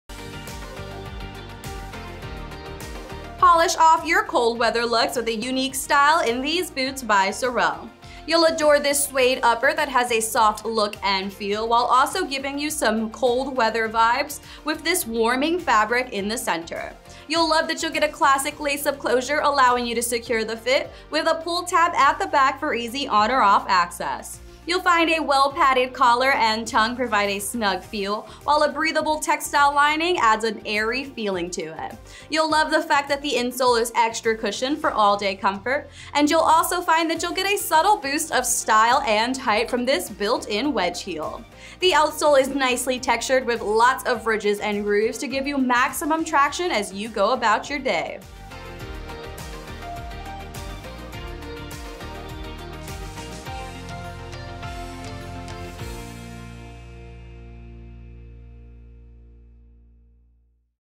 SOREL Evie™ Ankle Lace SKU: 9712652
Your wardrobe is no match for the chic and elegant style of the  SOREL&reg; Evie&#8482; Ankle Lace.
Lightweight without sacrificing comfort and performance.
Pull tab for easy on/off access.
Foam padding placed around your ankle collar &amp; under the tongue for an incredibly comfortable fit &amp; feel.
Traditional lace closure for a secure fit.
Generously cushioned footbed for all day comfort.
Athletic lightweight flexible midsole with impact cushioning protection.
Unique outsole design saves on weight.
Leather upper.
Textile lining and insole.
Synthetic sole.
Imported.